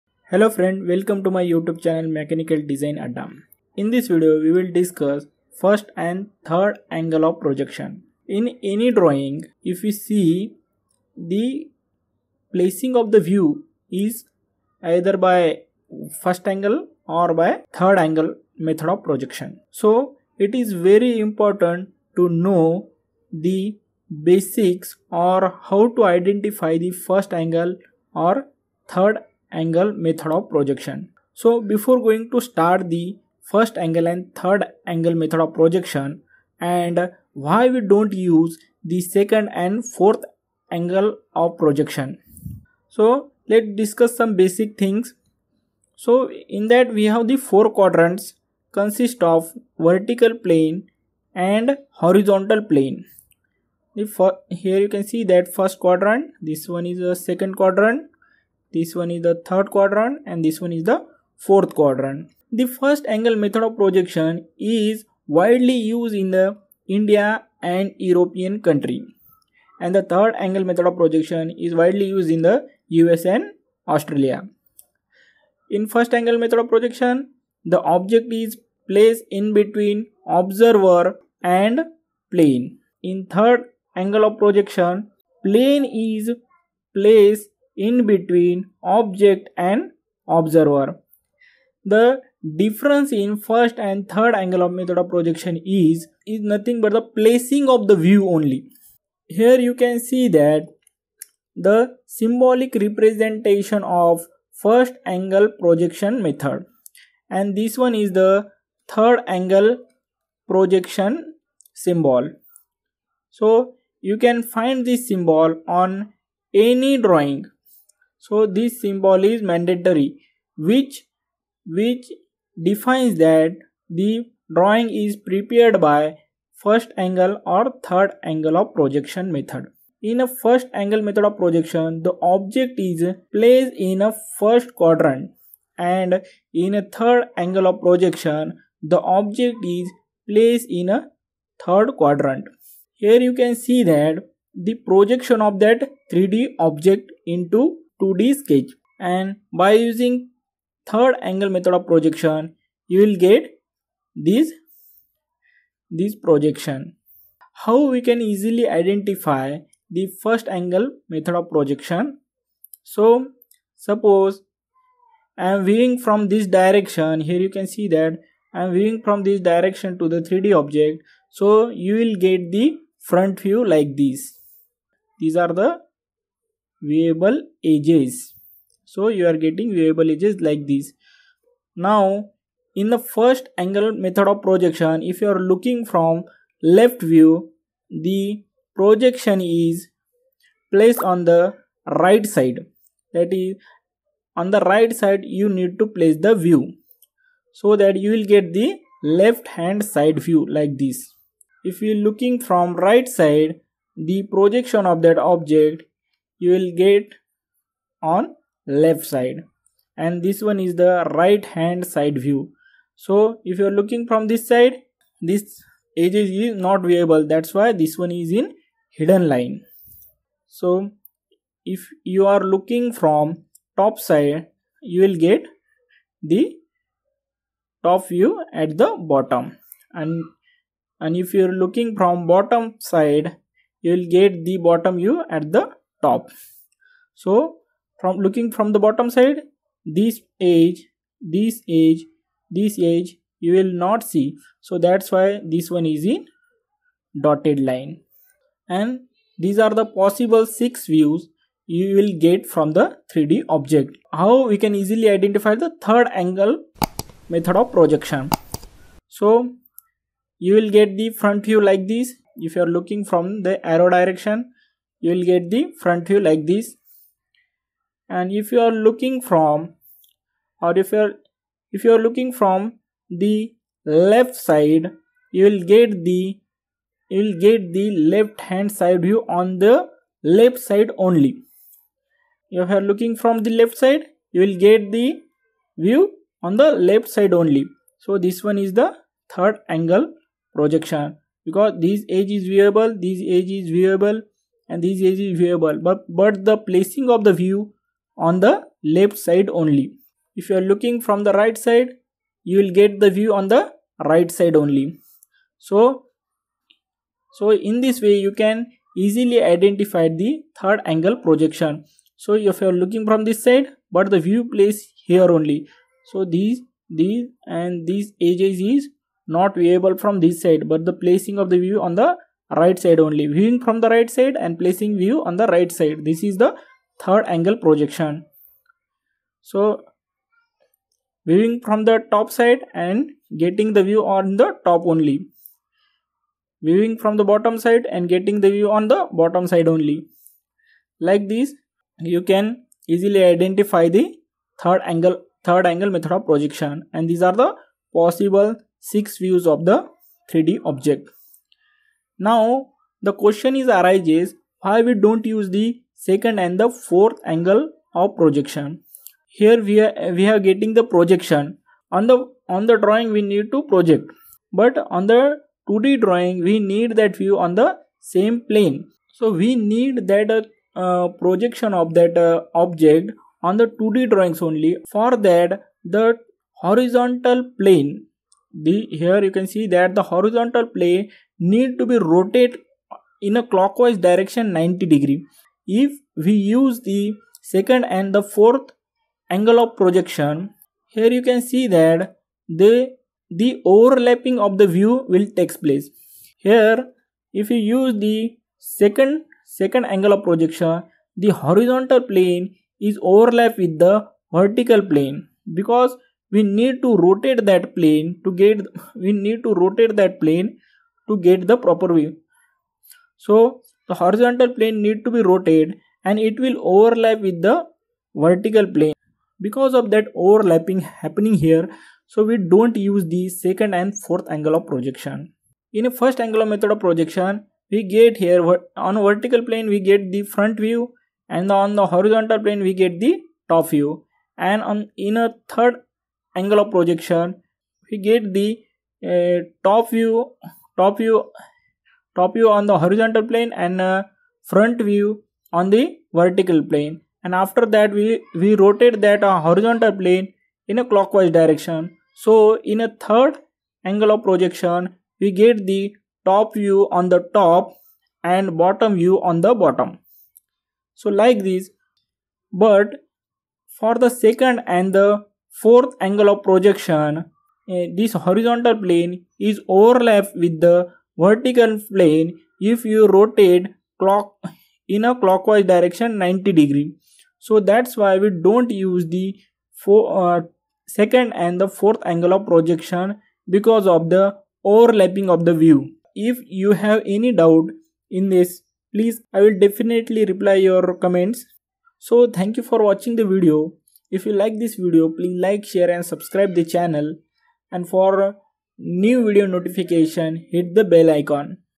Why 2nd and 4th angle projection not used ? | Difference between first and third angle of projection
After watching this video you will know below points.
1. what is first and third angle projection
2. Difference between first and third angle projection
3. How to easily identify first and third angle projection on drawing
4. Why we don"t used second and fourth angle projection

Mechanical Design Adda